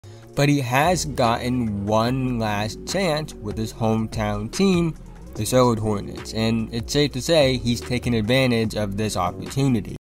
Dennis Smith Jr Is BACK
To say the Charlotte Hornets had a rough off-season would be an understatement. Between the legal issues among players on the roster (or were on the roster). And losing their 21 year old franchise player LaMelo Ball to an ankle injury at least for the start of the season. It hasn’t been the most ideal circumstances in Charlotte to say the least. And because of that they are projected to be one of the worst teams in the league. However to start the season I have actually watched more Hornets games than I ever anticipated. And that has to do with the play of their recent free agent signing Dennis Smith Jr. Who’s having a career resurgence.